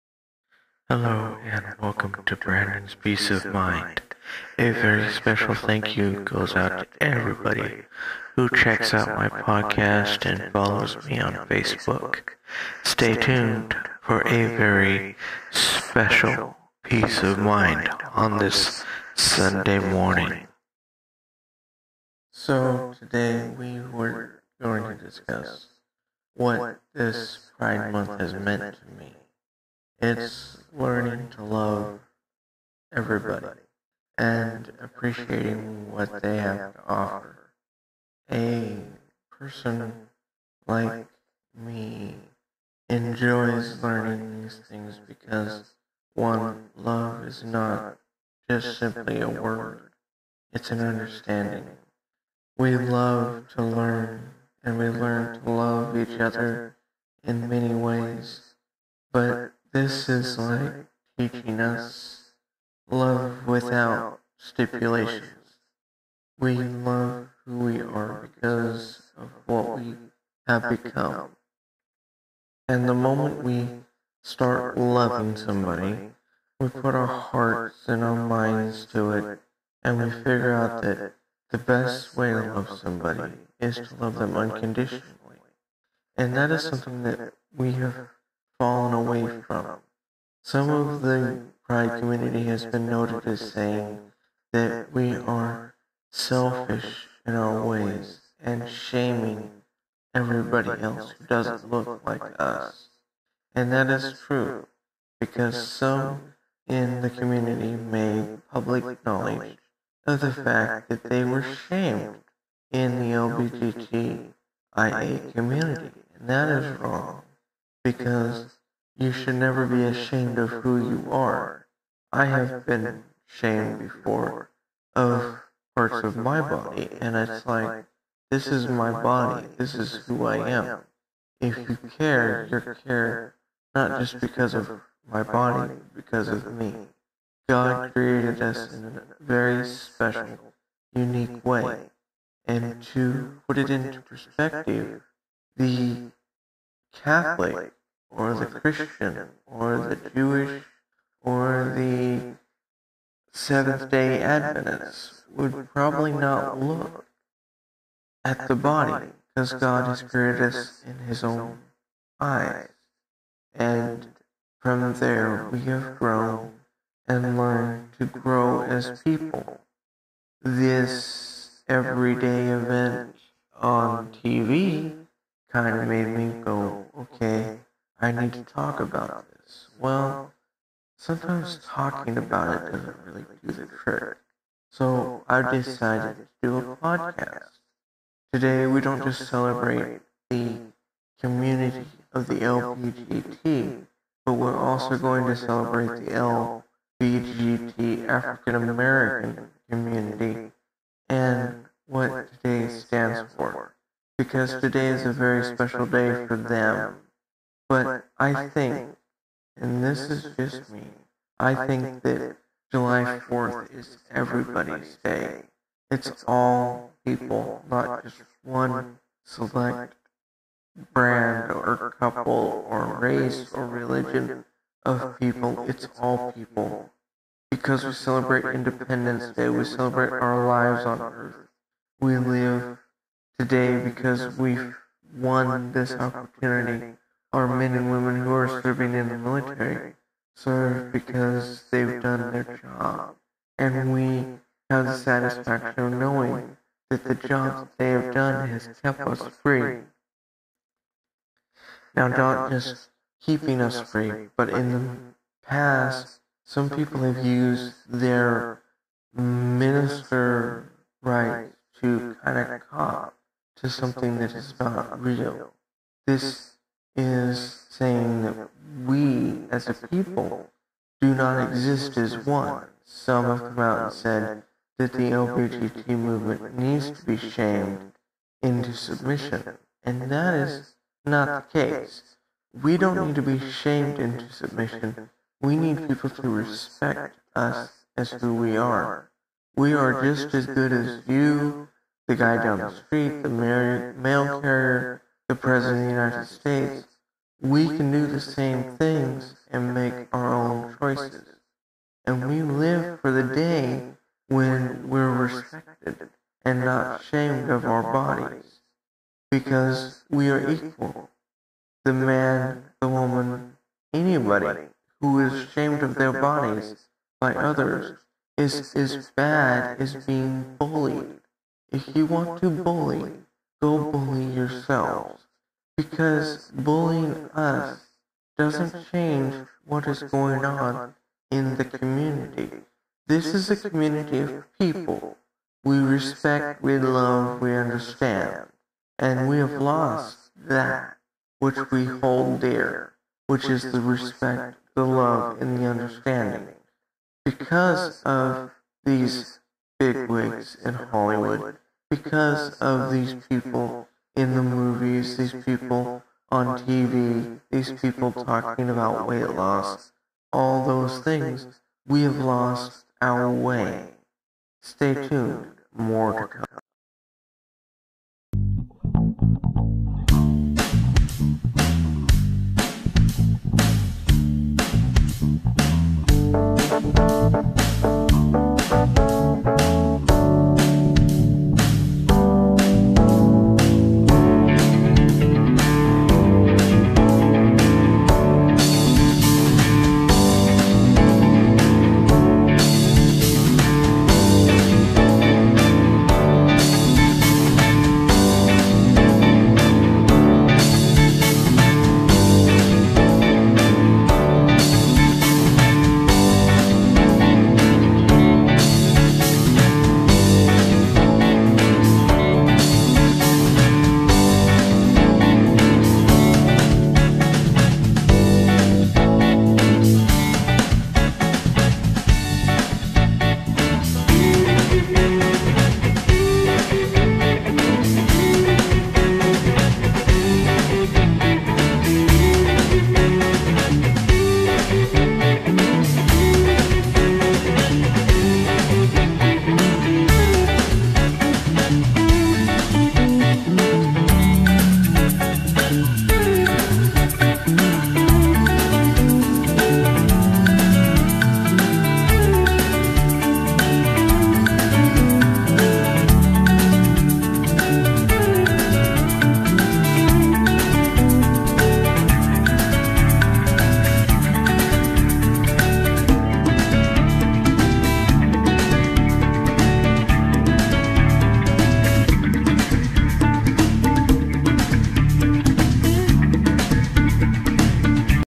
Sunday Peace of Mind by Brandon Leslie
so today I go on an adventure and I talk about what's on my mind. this is what I call peace of mind so you're about to get a piece of my mind and it's just something I like to do I created some music I've put it with this and just roll with it because there's a lot to talk about